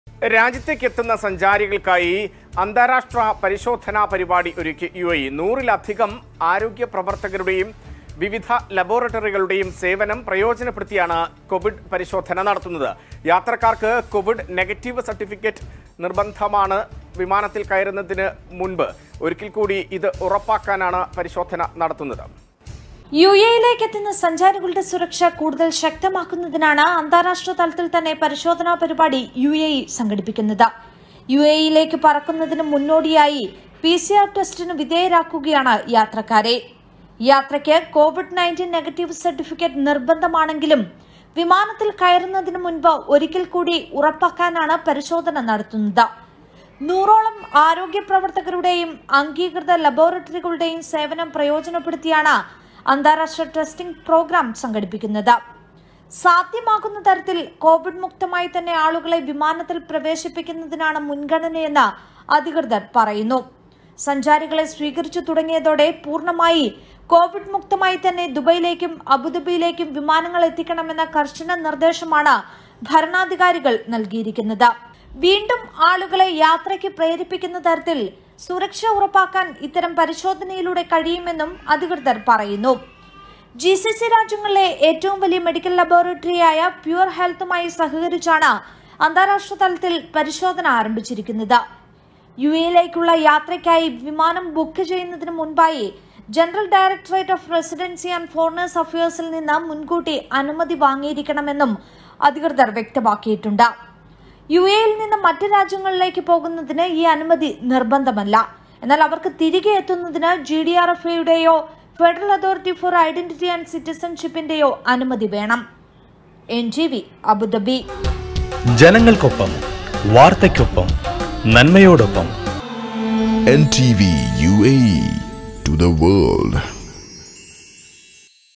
രാജ്യത്തേക്ക് എത്തുന്ന സഞ്ചാരികള്‍ക്കായി അന്താരാഷ്ട്ര പരിശോധനാ പരിപാടി ഒരുക്കി യുഎഇ.
രാജ്യത്തേക്ക് എത്തുന്ന സഞ്ചാരികള്‍ക്കായി അന്താരാഷ്ട്ര പരിശോധനാ പരിപാടി ഒരുക്കി യുഎഇ. നൂറിലധികം ആരോഗ്യപ്രവര്‍ത്തകരുടെയും വിവിധ ലബോറട്ടറികളുടെയും സേവനം പ്രയോജനപ്പെടു ത്തിയാണ് കോവിഡ് 19 പരിശോധന നടത്തുത്.
യാത്രക്ക് കോവിഡ് 19 നെഗറ്റീവ് സര്‍ട്ടിഫിക്കറ്റ് നിര്‍ബന്ധമാണെങ്കിലും വിമാനത്തില്‍ കയറുന്നതിന് മുന്‍പ് ഒരിക്കല്‍ക്കൂടി ഉറപ്പാക്കാനാണ് പരിശോധന നടത്തുന്നത്.